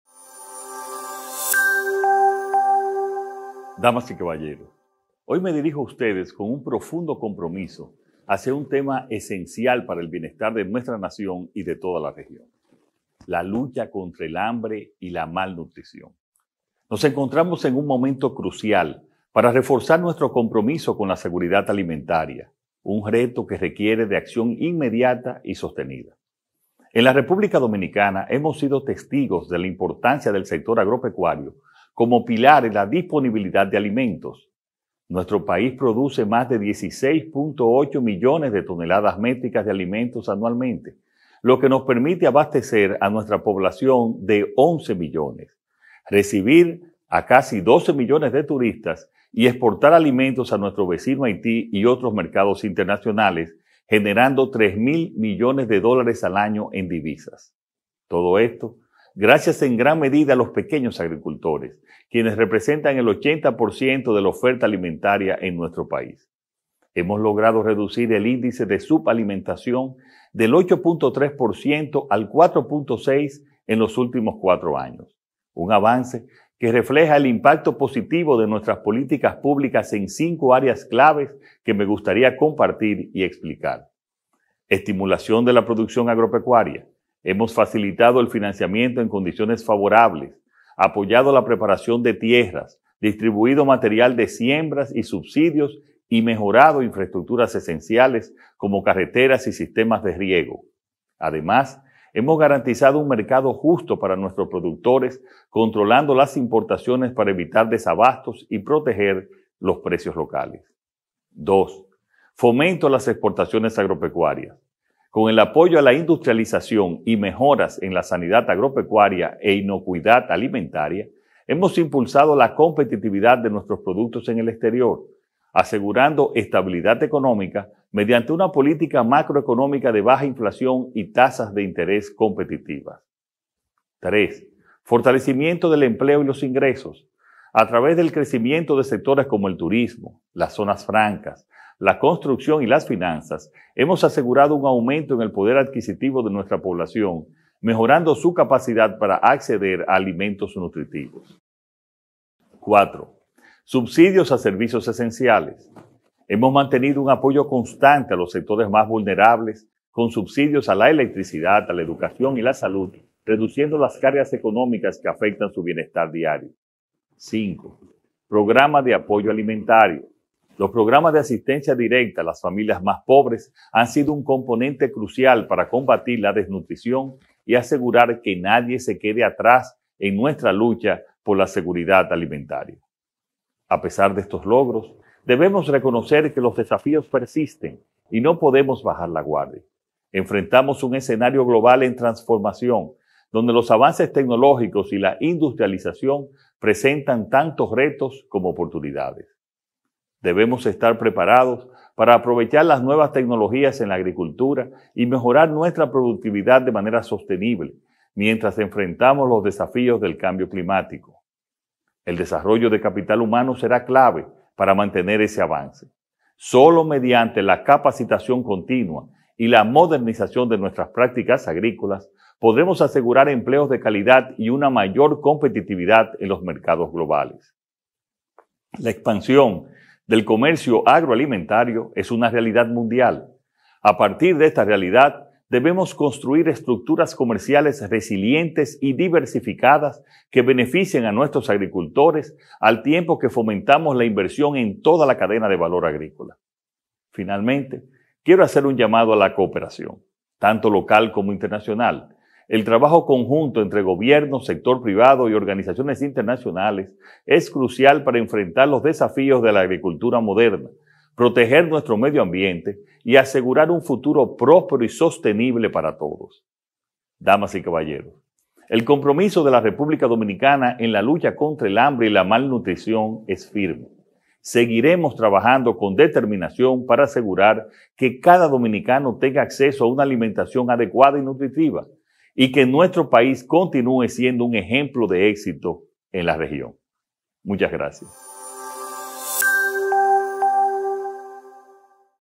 Luis Abinader, Presidente de la República Dominicana, en el Foro Mundial de la Alimentación
El acto principal de la cuarta edición del Foro Mundial de la Alimentación (FMA), el mayor encuentro anual de partes interesadas en los sistemas agroalimentarios a escala mundial, ha empezado hoy con una ceremonia de alto nivel en la Sede de la Organización de las Naciones Unidas para la Alimentación y la Agricultura (FAO) en Roma. Del 14 al 18 de octubre, el acto reunió a expertos mundiales, jóvenes agentes del cambio, inversores y líderes visionarios con el fin de crear nuevas vías para un futuro sostenible, inclusivo y con seguridad alimentaria para todos.